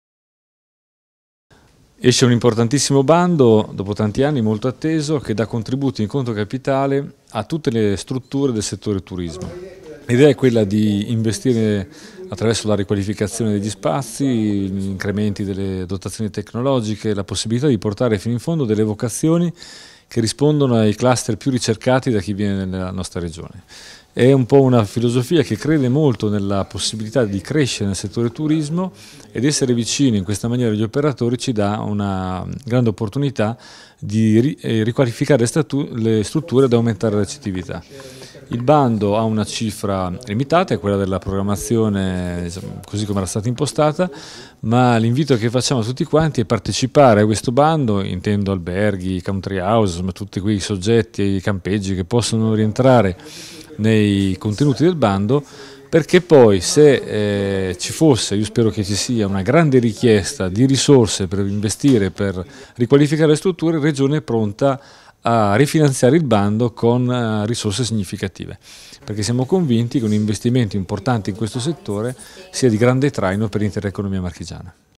Turismo: un bando per promuovere la competitività delle piccole e medie imprese
Ceriscioli: "pronti a rifinanziare il bando con risorse significative"